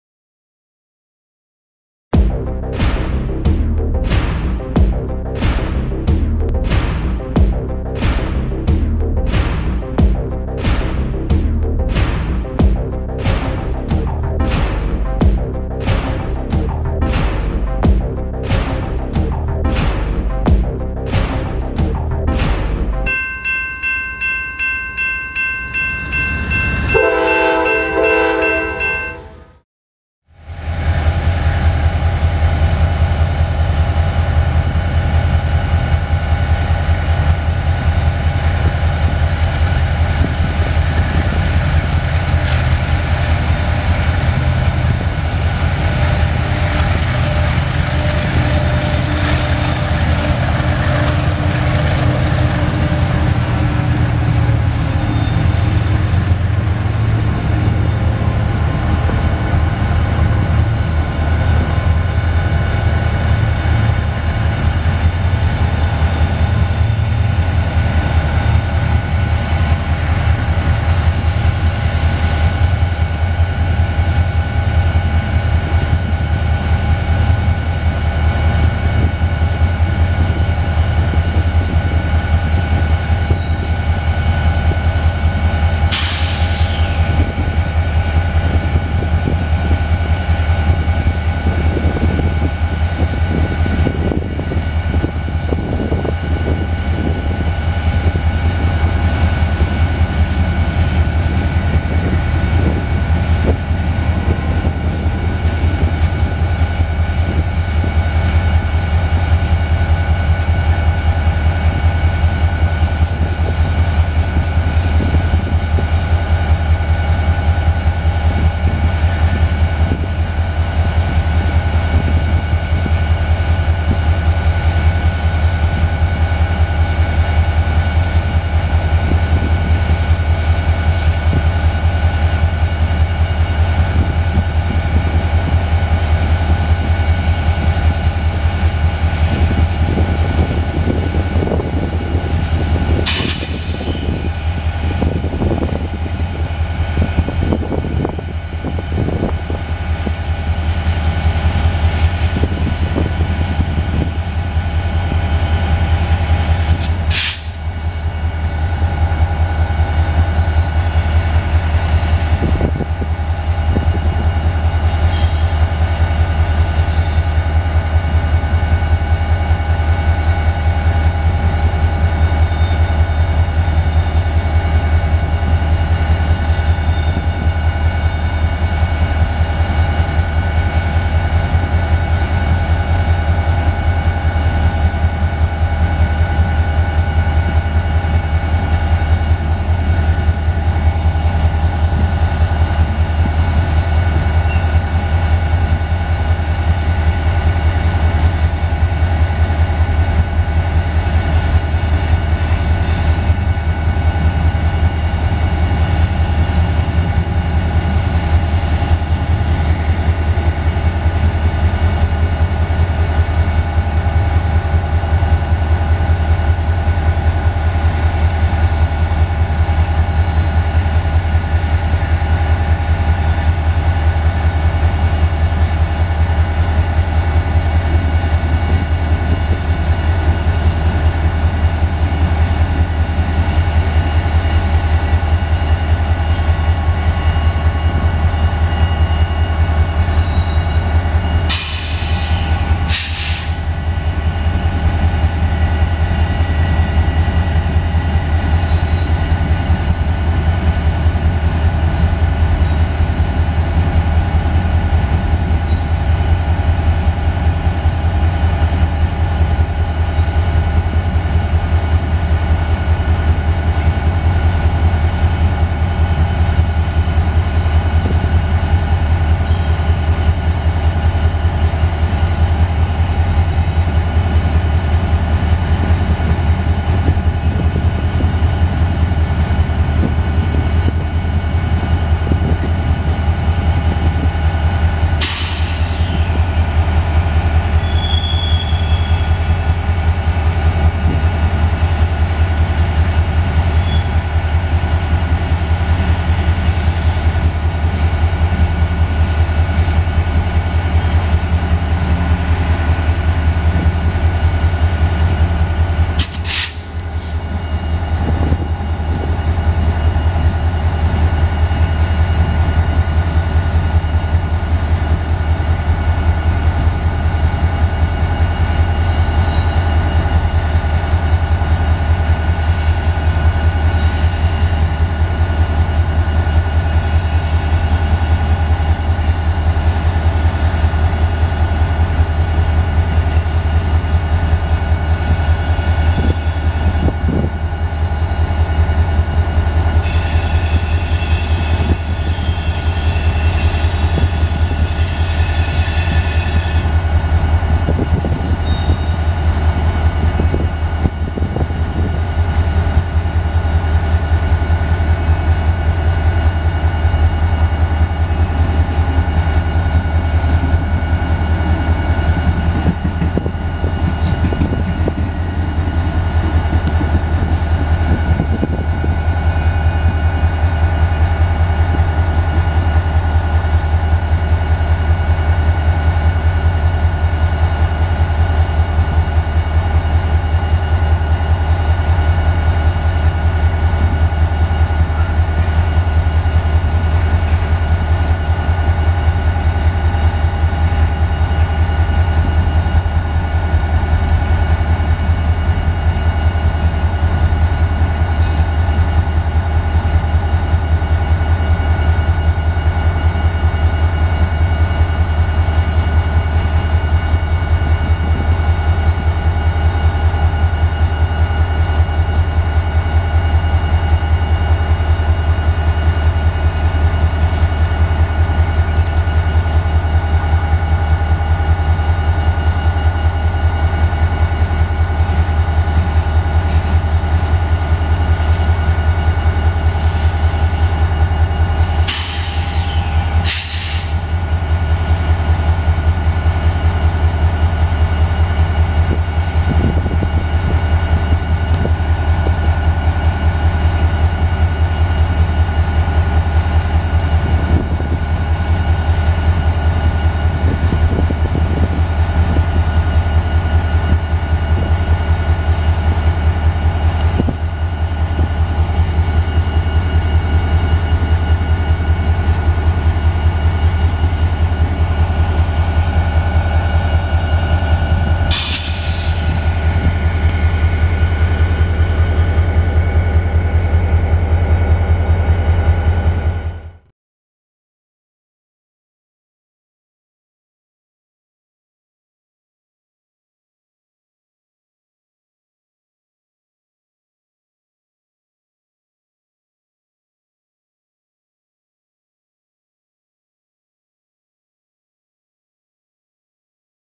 Train Engines Galore at ADM in Evansville, Indiana
As CSX 6142 sits loudly idling while waiting to locally deliver some covered hoppers, a 5-engine CSX mixed freight train heads south toward Howell Yard. The 96-car train is powered by CSX 3335, CSX 973, CSX 7208, CSX 3444, and CSX 885. The black engine on the closest track near the signal is an Archer Daniels Midland switcher, ADMX 57. Recorded near ADM in Evansville, Indiana, on May 3, 2020.